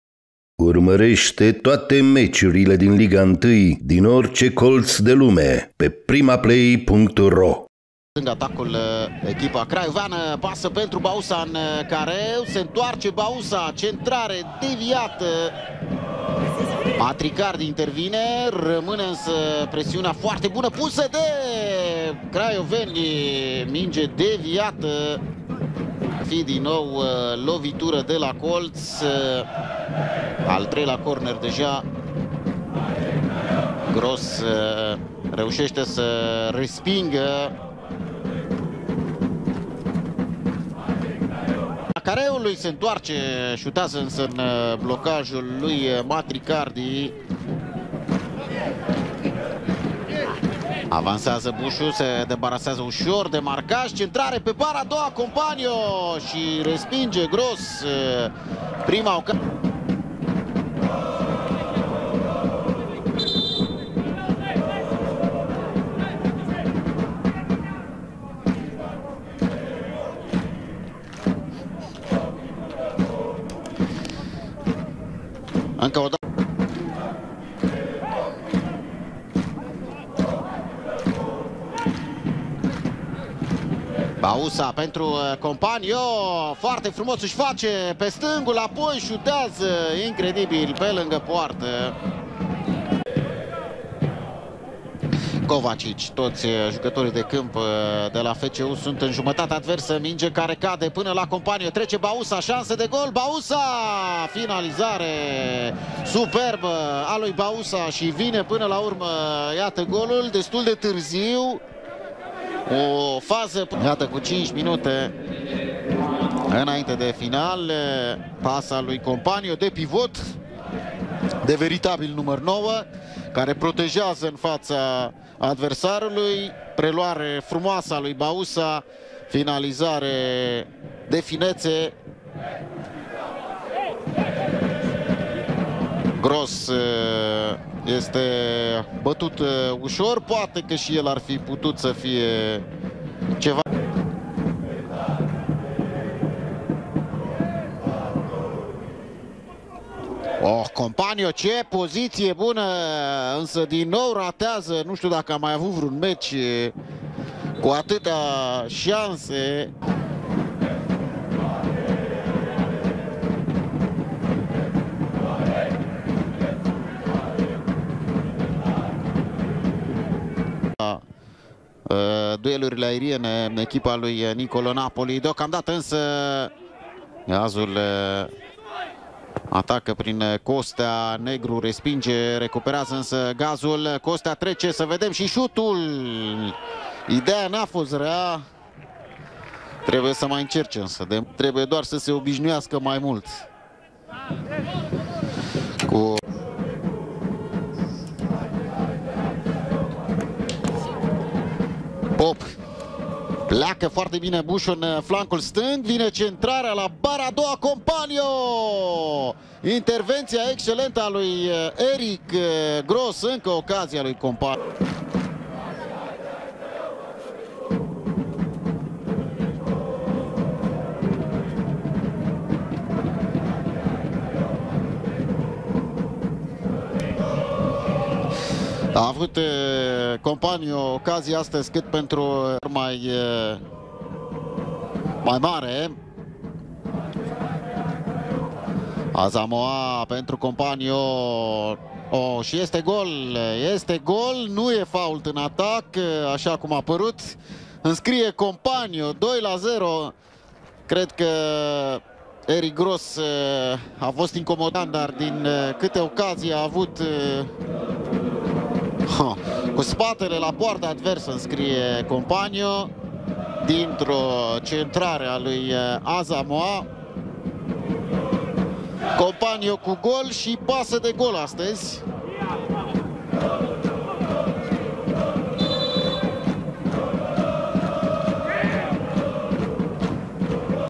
REZUMAT | Gaz Metan Mediaș – FC ”U” Craiova 0-2 | Play-out, Etapa 2, Liga 1, 2021-2022
Au marcat: Bauza '40,  Compagno '81.

Au evoluat echipele:

Gaz Metan: Grosz – Kanda, Matricardi (Tala '46), Avram, Maxim – Harfaş (Covaci '46), P.Iancu – Ursu, Gaşpar (Câmpean '84), P.Costea – Domşa (Noian '46); Antrenor: Flavius Boroncoi

FC U Craiova: Popa – Negru, Diallo (Paramatti '84), Kovacic, Buşu – Asamoah, Marquet (Pop '66)– Baeten (Blidar '84), Bauza (Bălan '66), Van Durmen (Sidibe '46) - Compagno; Antrenor: Nicolo Napoli